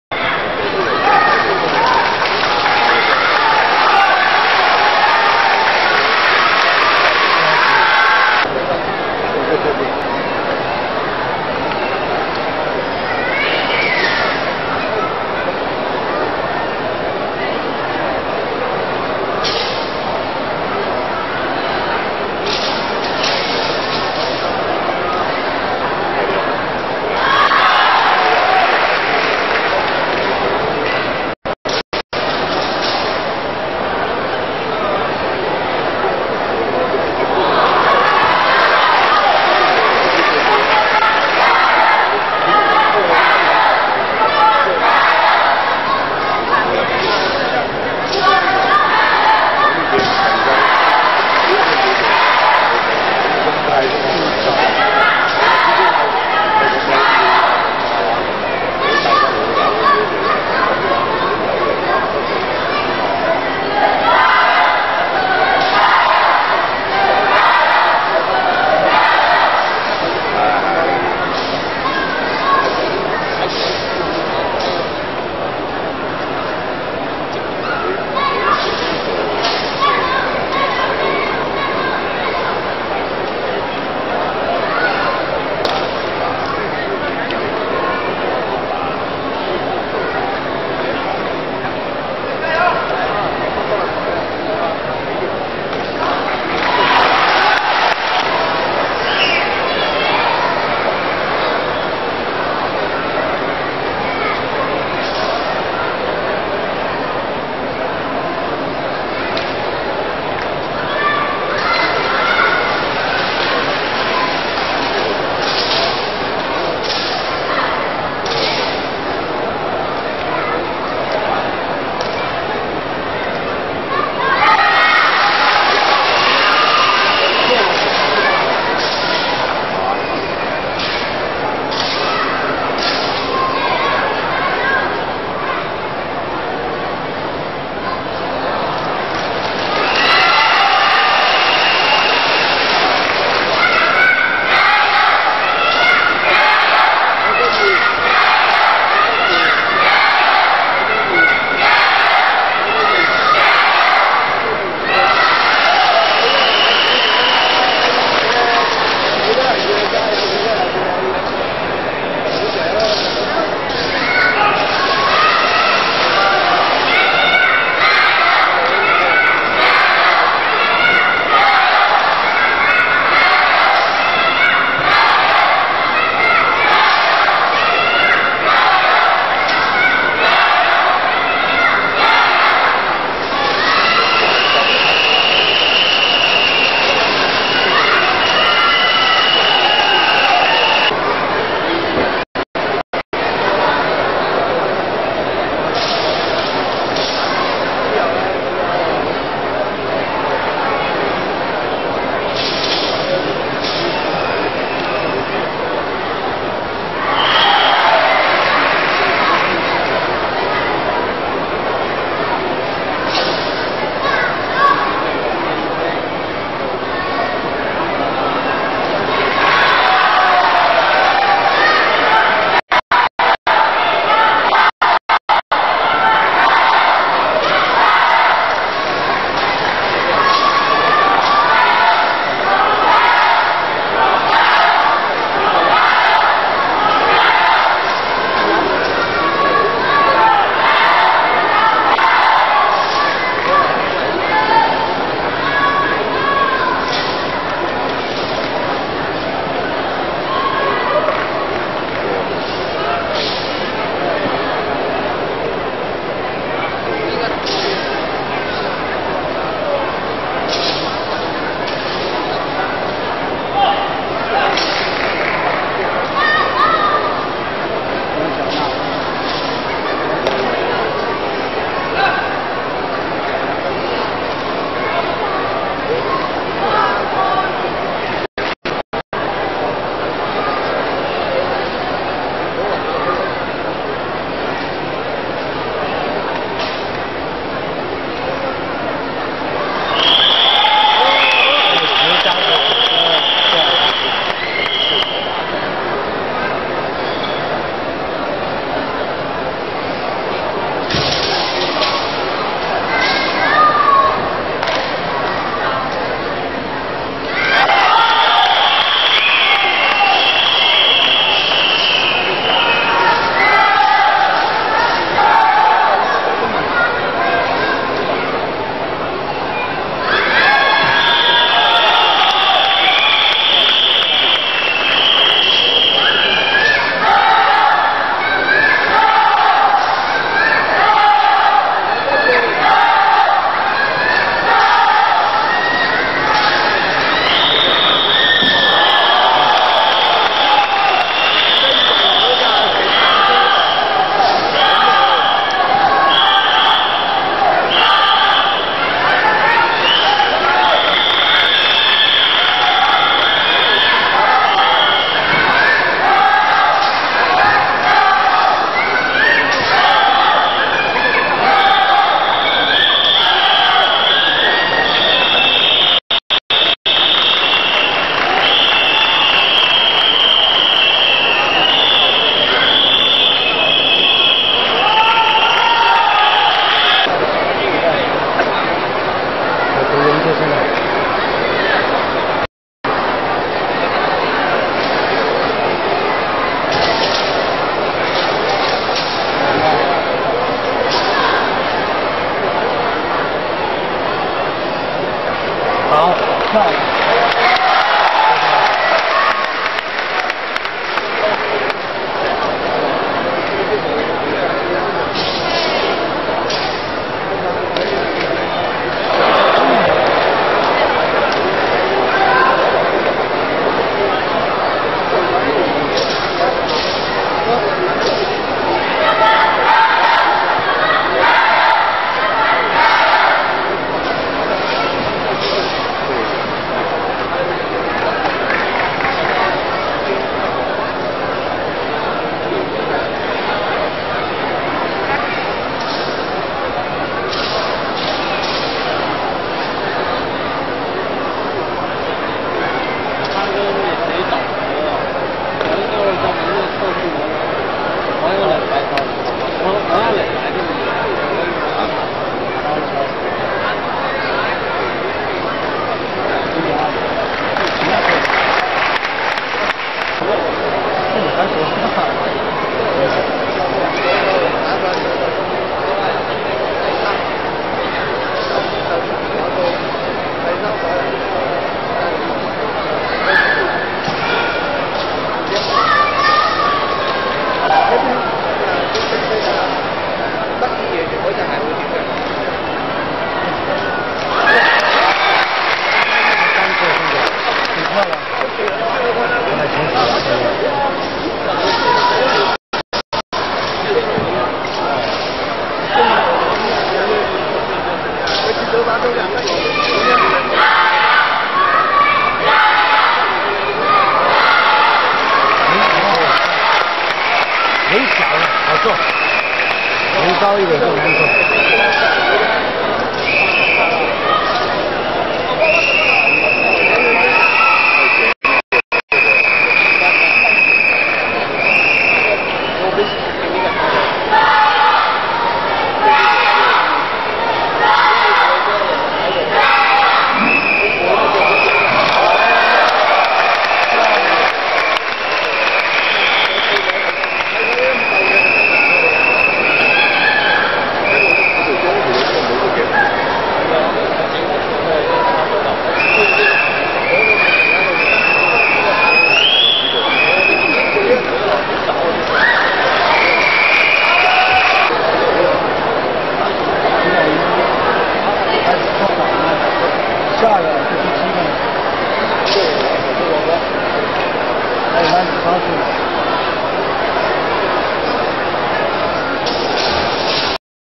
9th All China Games Women's Individual Daoshu Part 1
Part 1 of 2. I'm clueless about the women of Daoshu, but they're all pretty damn good. Enjoy.